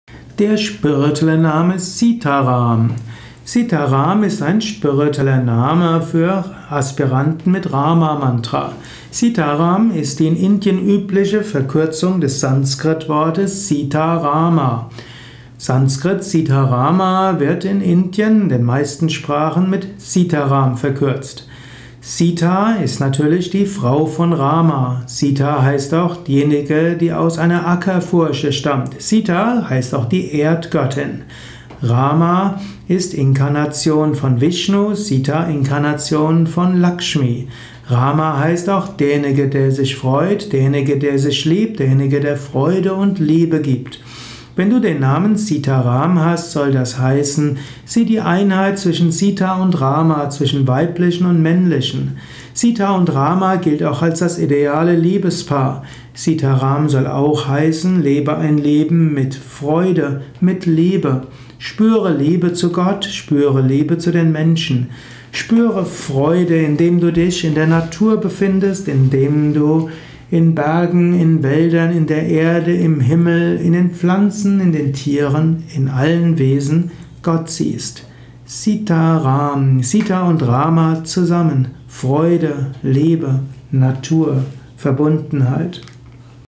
Sitaram als Spiritueller Name - Übersetzung aus dem Sanskrit und Erläuterung
Was ist die Bedeutung von Sitaram als Sanskrit Name, als indischer Name, als spiritueller Name? Sitaram Rama mit Sita, im Unterschied zu Parashurama (Rama mit der Axt, 6. Inkarnation Vishnus) und Balarama (Kraft-Rama, Krishnas Bruder). Sitaram ist die Hindi Verkürzung des Sanskrit Wortes Sitarama. Sitaram ist ein indischer Name. Wenn du also jemanden aus Indien kennst, der/die diesen Namen hat, dann kannst du hier erfahren, was dieser Name bedeuten kann .Dies ist auch ein Namen, der im Westen Yoga Aspiranten als spiritueller Name gegeben werden kann. Sitaram wird insbesondere vergeben an Aspiranten Rama Mantra. Wenn du Sitaram als spirituellen Namen hast, bekommst du ein paar Anregungen, was dieser Name für dich bedeuten könnte.

Hier in Kürze die Zusammenfassung: Sitaram, Sanskrit सीताराम sītārāma m., Rama mit Sita, im Unterschied zu Parashurama (Rama mit der Axt, 6. Inkarnation Vishnus) und Balarama (Kraft-Rama, Krishnas Bruder). Sitaram ist die Hindi Verkürzung des Sanskrit Wortes Sitarama.